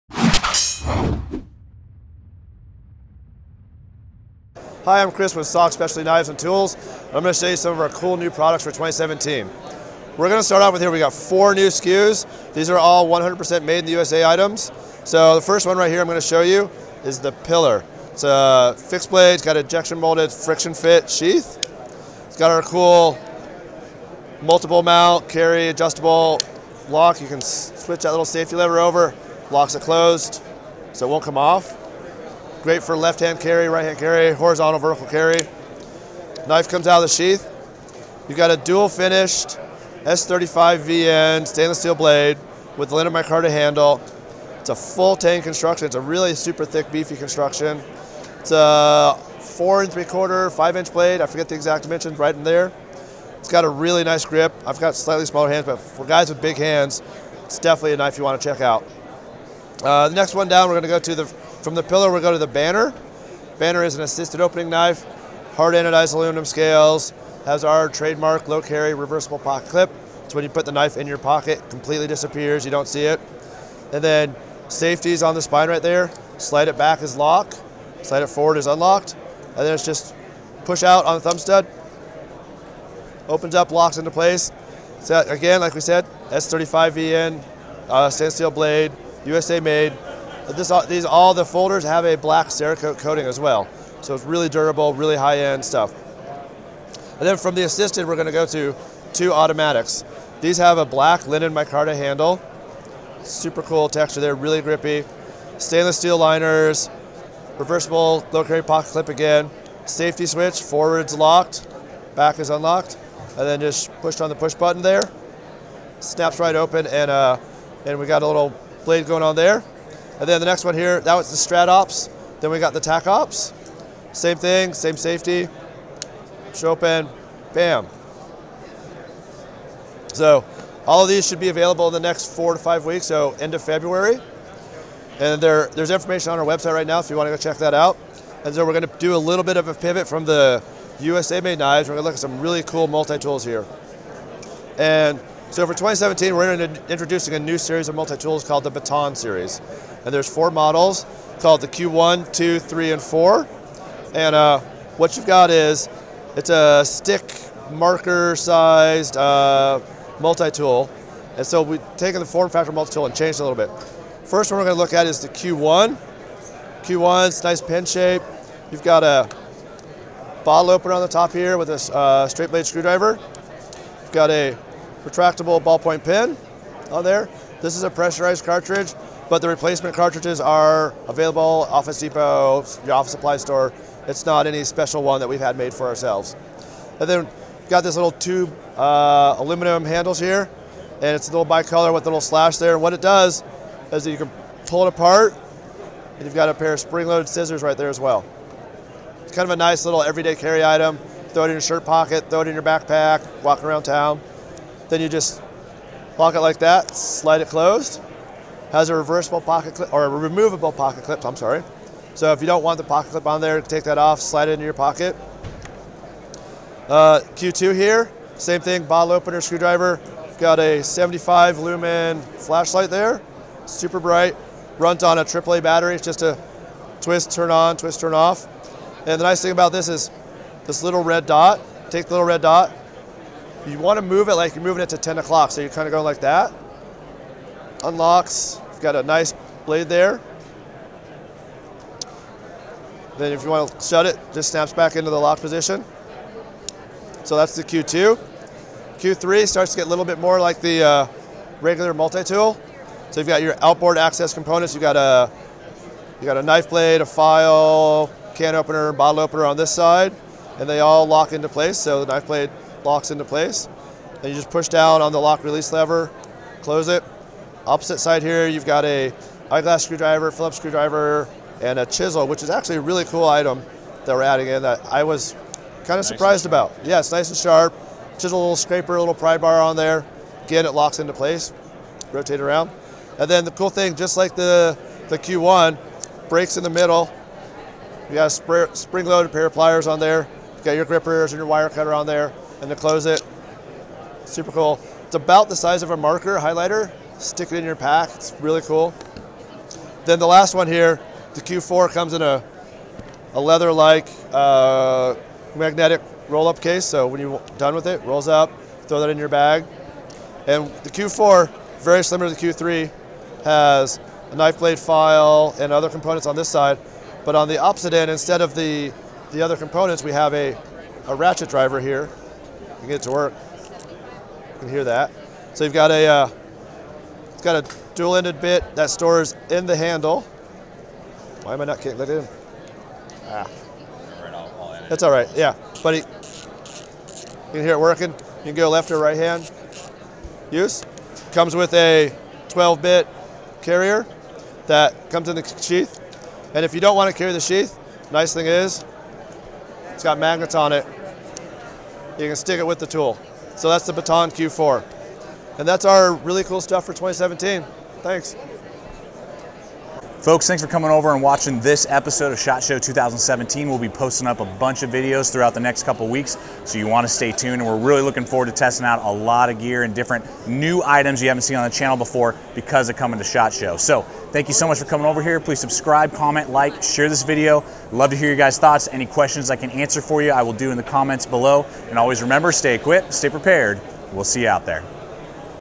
Shot Show 2017: SOG USA Made Knives + Q Multitools
Here at shot show 2017 I had to hit the SOG booth because of their big news that hey are make some USA made s35vn knives and also came out with a new multitool design!